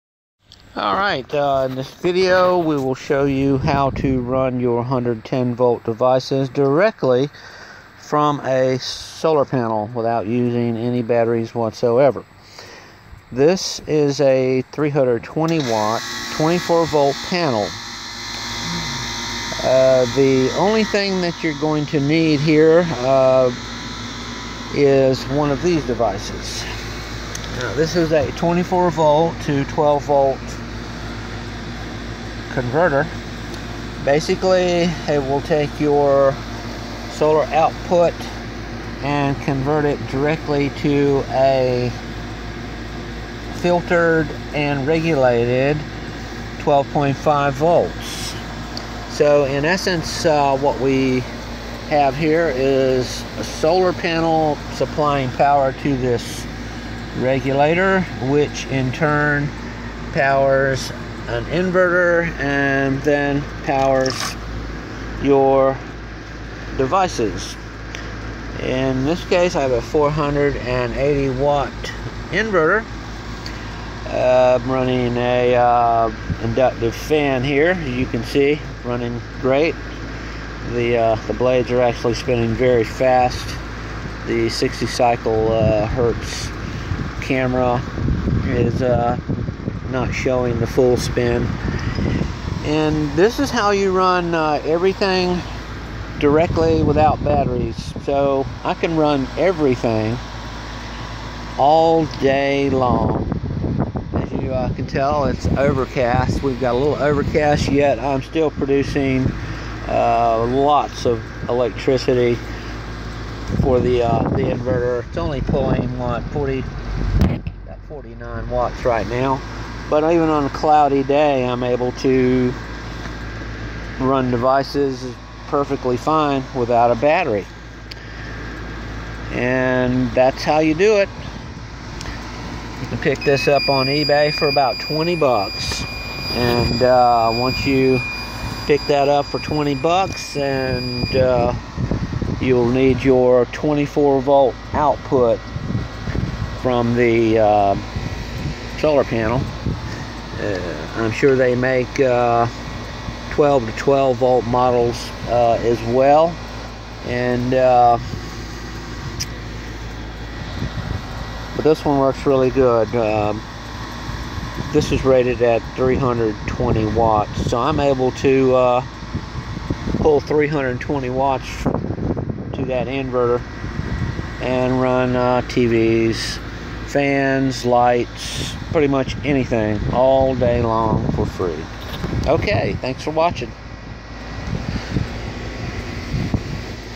Use inverter on solar panel without battery
This is a way to use 115vac appliances directly from a solar panel. I am using a 320watt 24vdc panel connected to a 470watt 115vac out inverter.surge is supposed to be 800watts. It requires a 12 volt dc input. This inverter is very picky about voltage input. It has a digital readout and displays errors if voltage is too low or below 10.7vdc or error voltage too high 15.8 and above. Either condition shuts the inverter off and sounds an alarm. I hooked it up directly to the 24 volt output of my panel and the inverter started smoking and died.luckily im a repair technician and found that several large diodes were burnt and replacing them brought the inverter back to life. Ok so here is what you need to connect a large panel to an inverter without a battery as a buffer. This is a 24 to 12 volt dc to dc converter. Its cheap and cost me 20dollars and rated at 375watts. It provides a steady regulated 12.5 volts output no matter how much sun is shining on the panel. It works perfectly and as long as the sun is shining and its daylight i can run a satellite and tv system ,4 bright led lamps with a light rating comparable to 60watt incandescent  (led light is much more brighter and the light spectrum i that of the sun)and a 70watt metal fan 3speed all for free all day long.. . Gt one it is well worth the money and no batteries needed at all for ac power during the day..